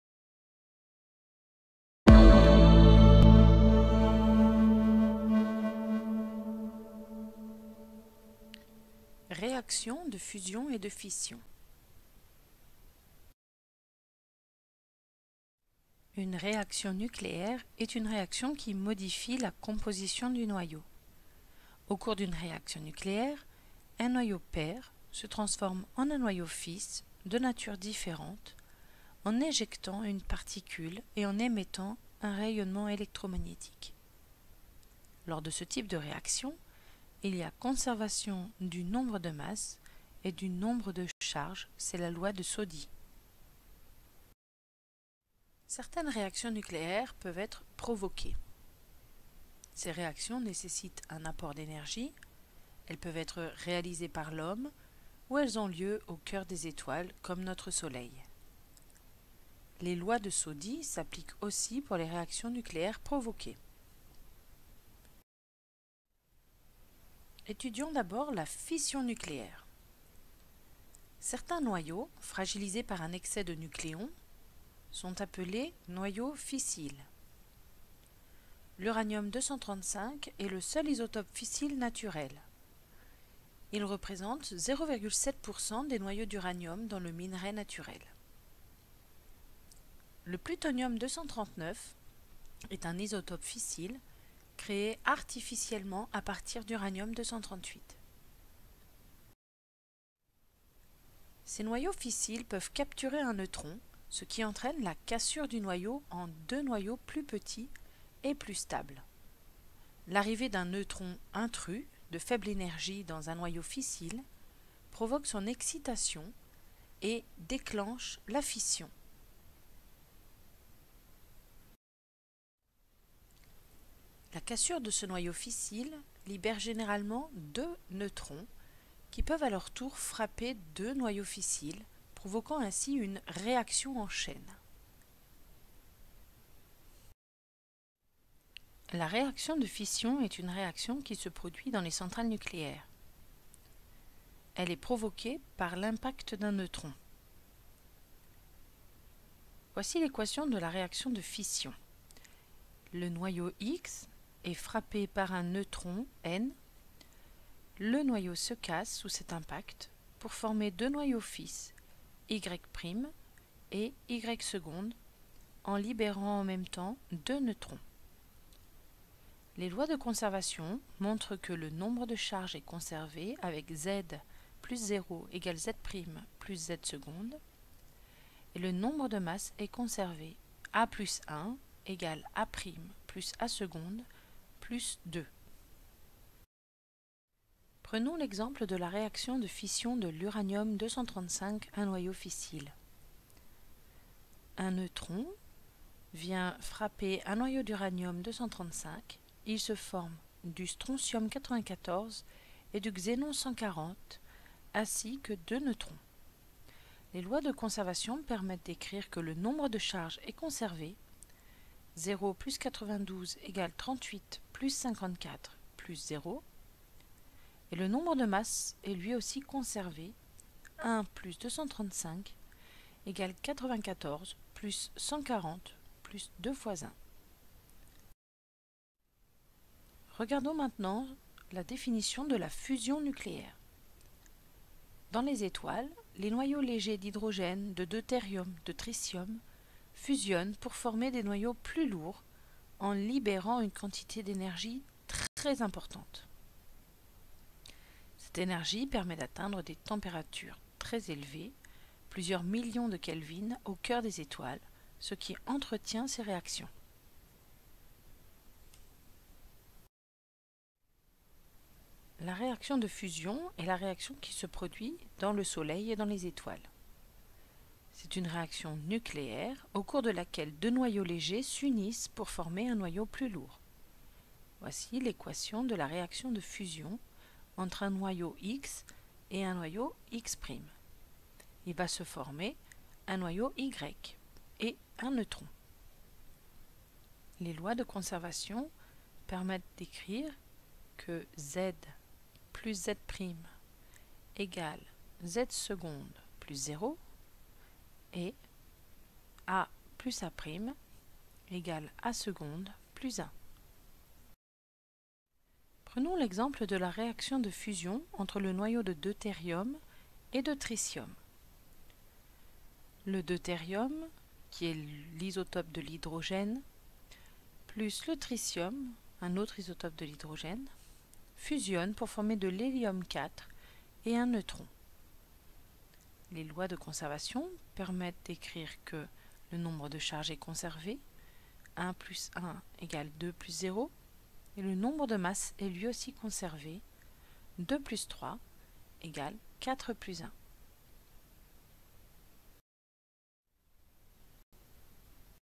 Réactions de fusion et de fission
Définition et exemples de réactions de fusion et de fission, les deux réactions nucléaires provoquées.